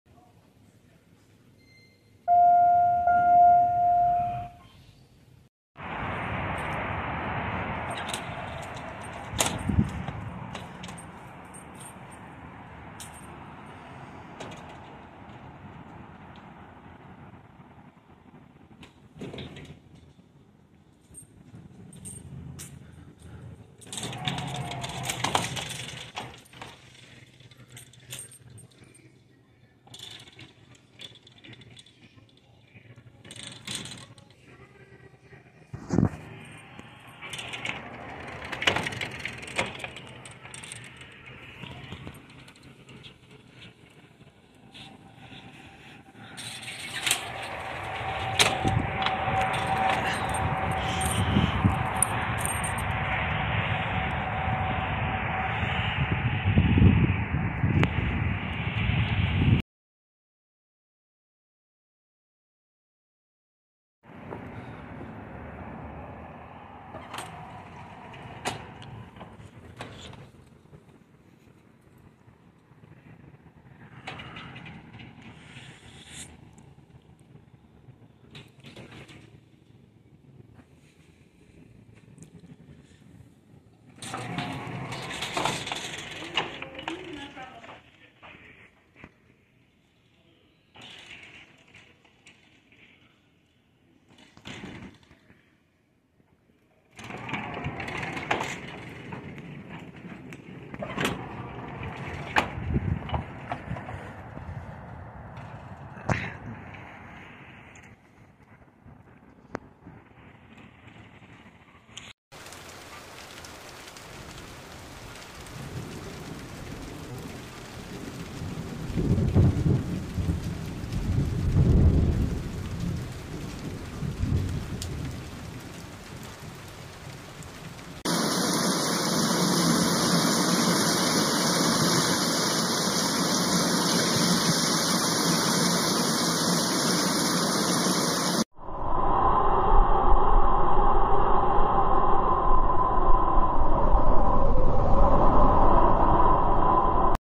(2023 - VERY RARE) Stanley Dura-Fit Automatic Doors - Comfort Suites - Green Bay, Wi.
Filmed on Wednesday, November 22, 2023

This kind of door is very rare. It is a very interesting door, when I first stayed here. I was like "What the heck are these doors?!" Revolving Doors aren't very popular either, they put in sliding doors a lot now.